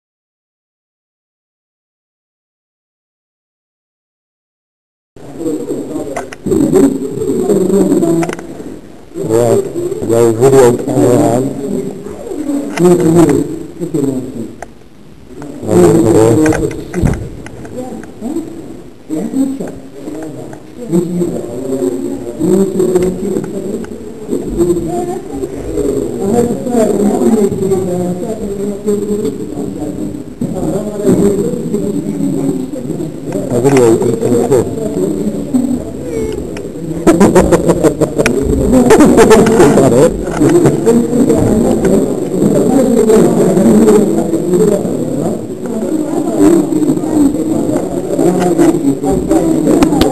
Gemma stuffing face 06/11/2009
This is the video Gemma requested, see her stuffing her face with junk.